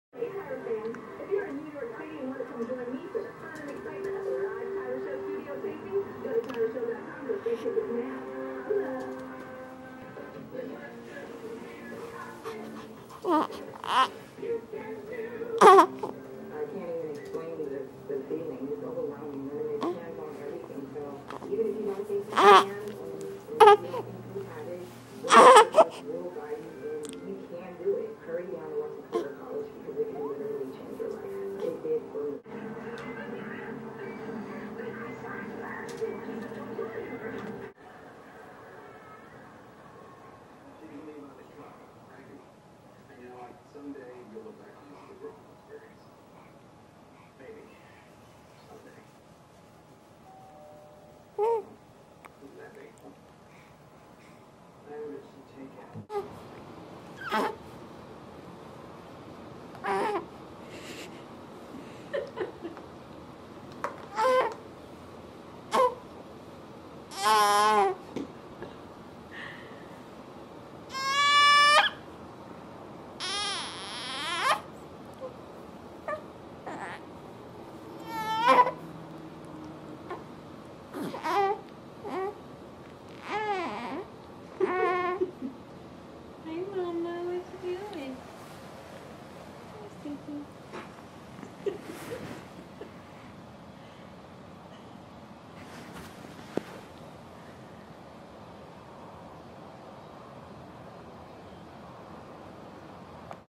Sleeping Siena Montage
Baby girl's paparazzi takes lots of video of her while she sleeps.